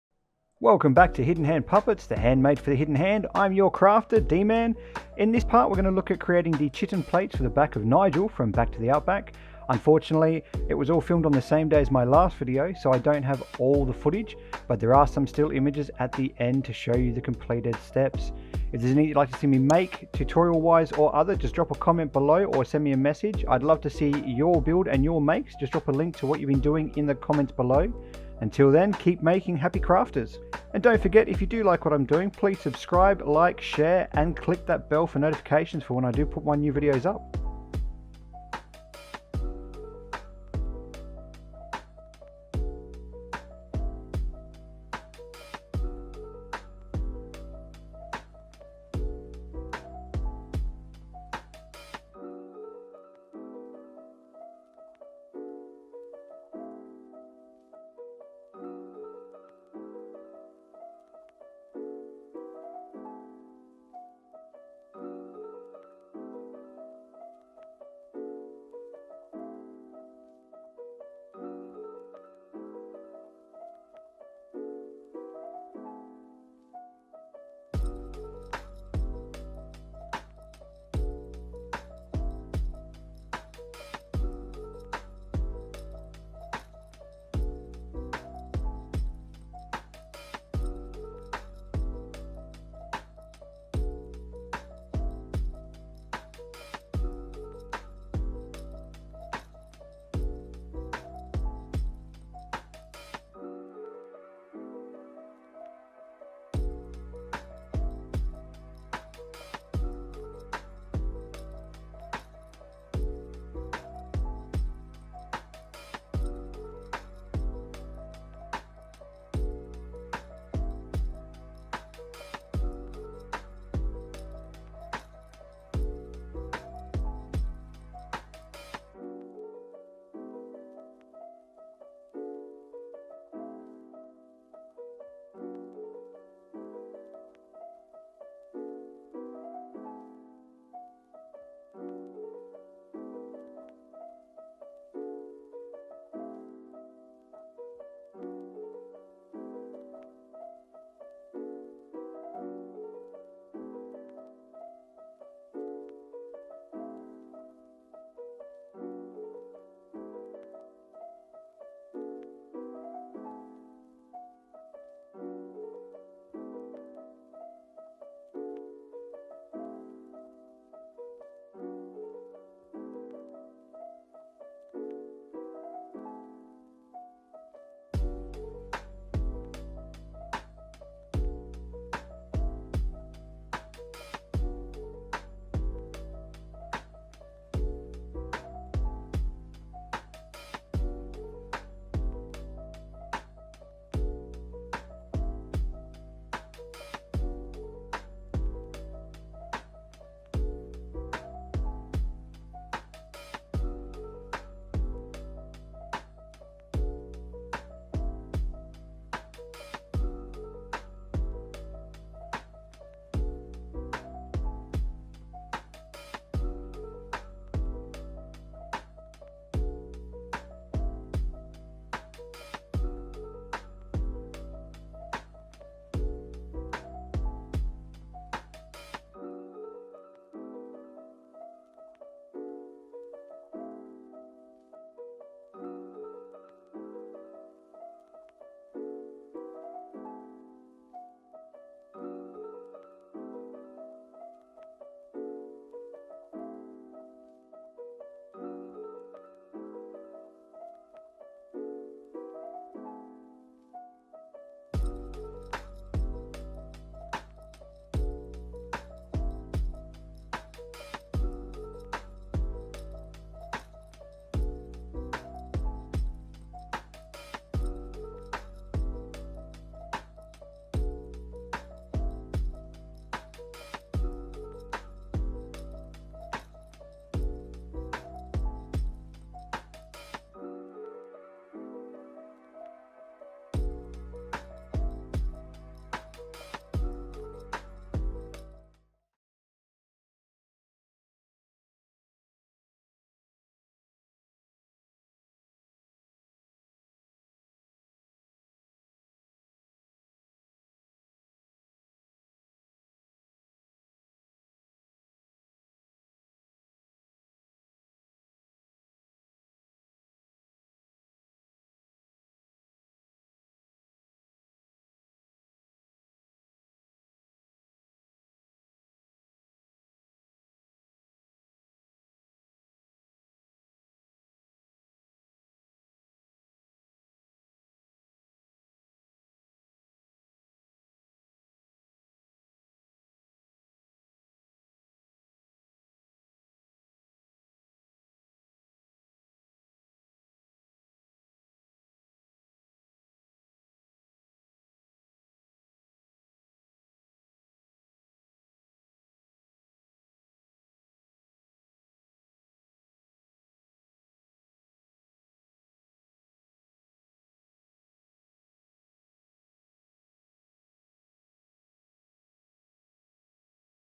Time lapse Puppet Build Nigel Pt 7: Back plates - Chitin
Here we go folks welcome to Pt 7 of my Time Lapse build of Nigel from Back to the Outback , this is where we start the beginnings of the details, this is where Nigel will begin taking shape.  In this video I design and make the back plates or Chitin plates as they are known.

I used 6mm HD eva foam, cut a "V" and glued the sides of the V together to create the "Ridge" in the plates, then I made the felt covers for them and hand stitched them over the foam.

Lastly I wanted to test out the airbrush color on some scrap yellow fleece before painting the plates. I love to overall look. Now to make the same paint color ;)